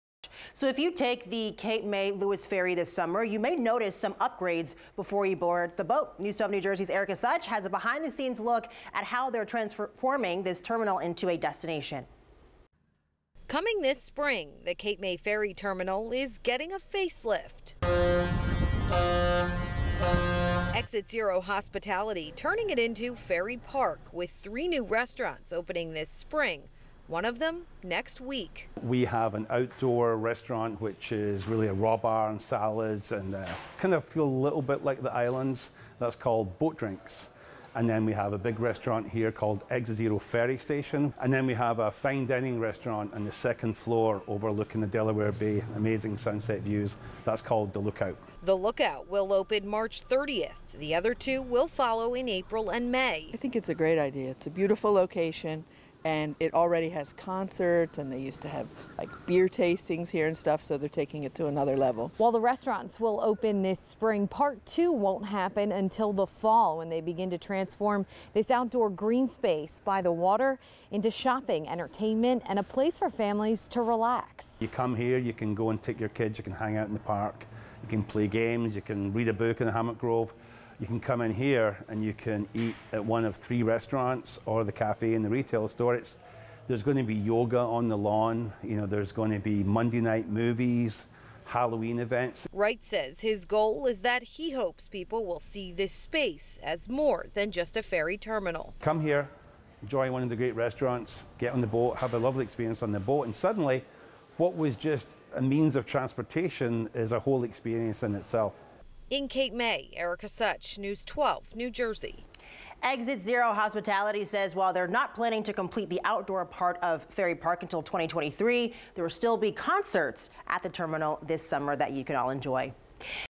Cape May-Lewes Ferry Terminal to undergo major upgrades
The Cape May-Lewes Ferry Terminal will be getting some upgrades this year. Exit Zero Hospitality is turning it into Ferry Park, with three new restaurants that are opening this spring.